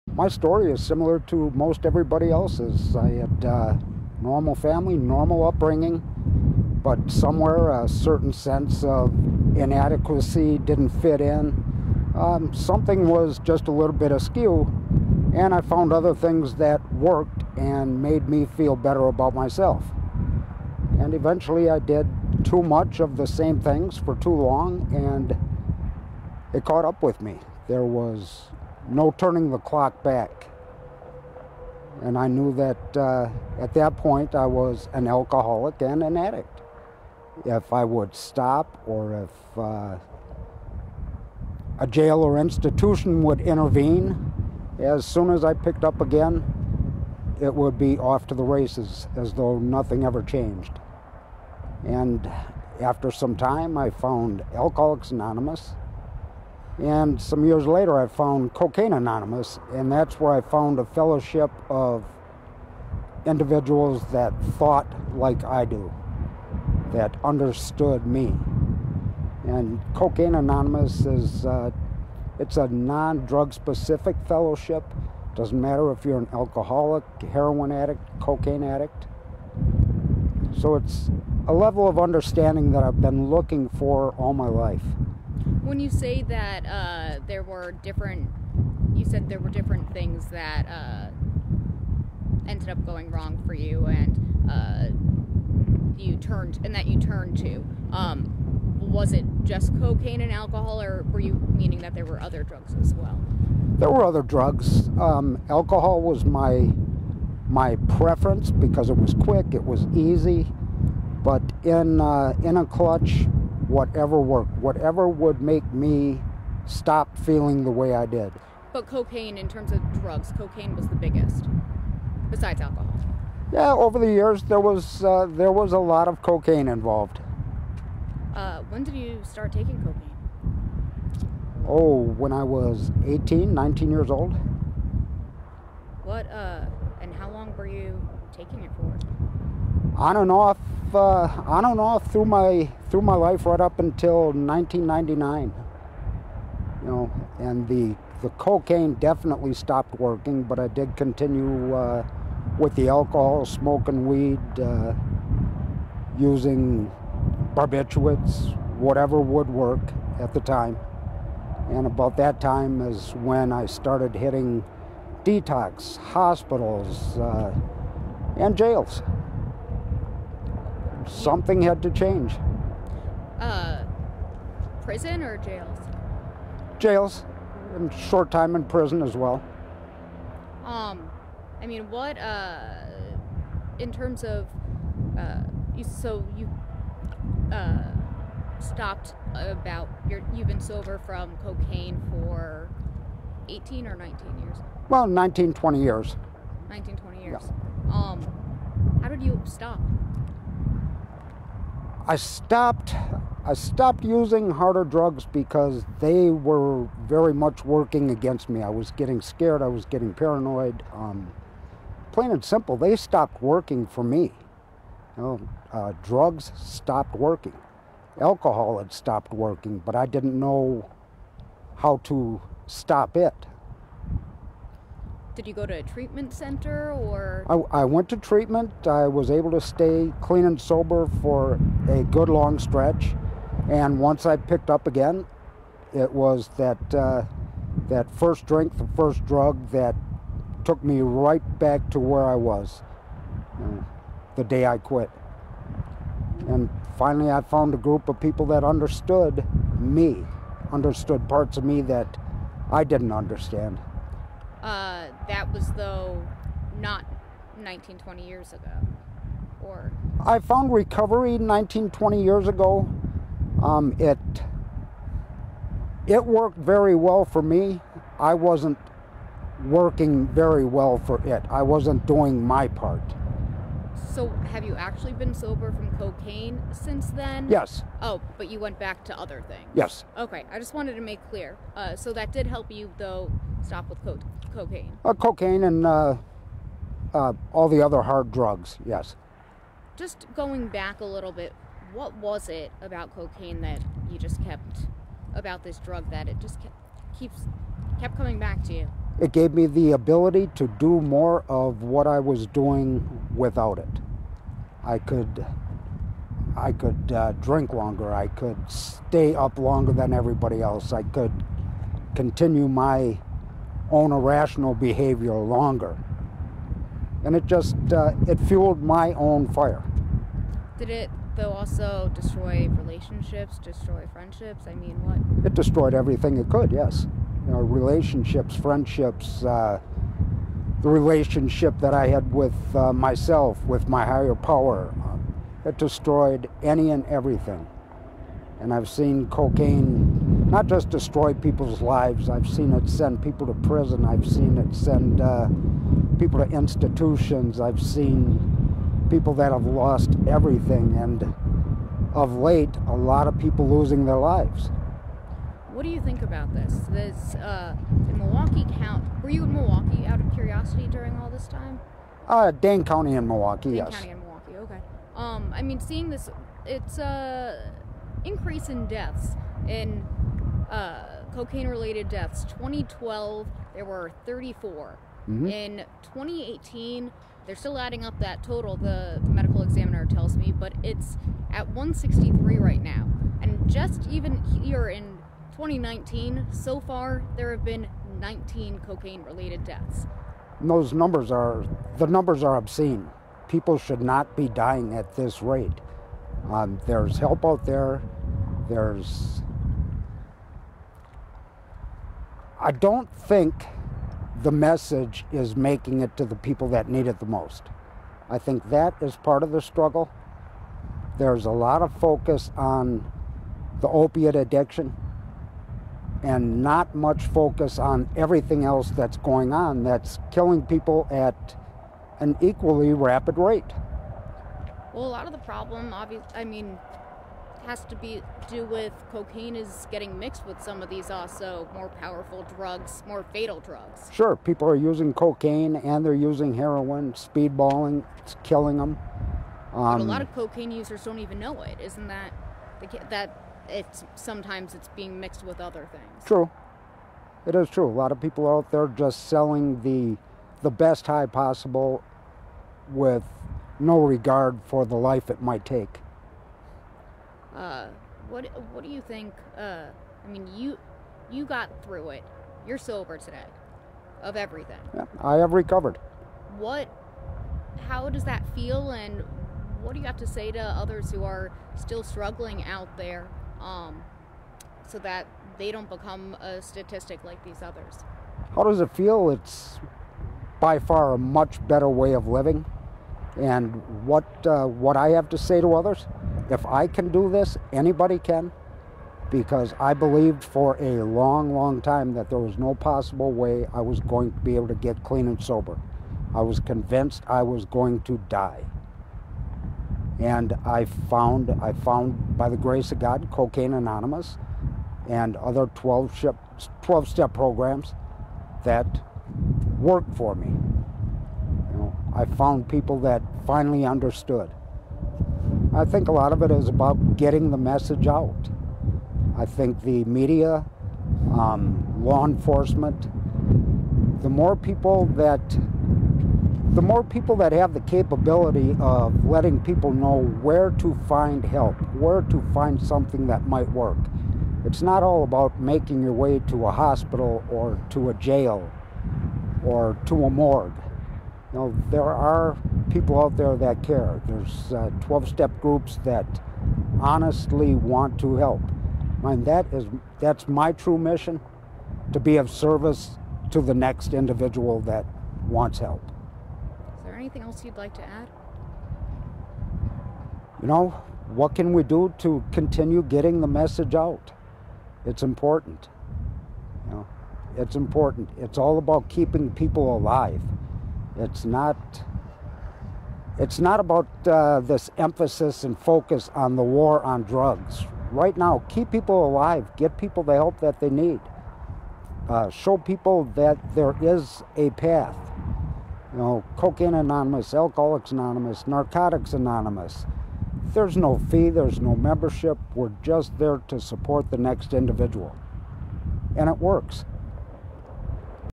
RAW INTERVIEW: A former cocaine addict describes how he got clean
Cocaine deaths are on the rise - both around the country, and here in Wisconsin. Lauren Linder spoke with one man who kicked the habit years ago, and he has tips for others who want to follow in his footsteps.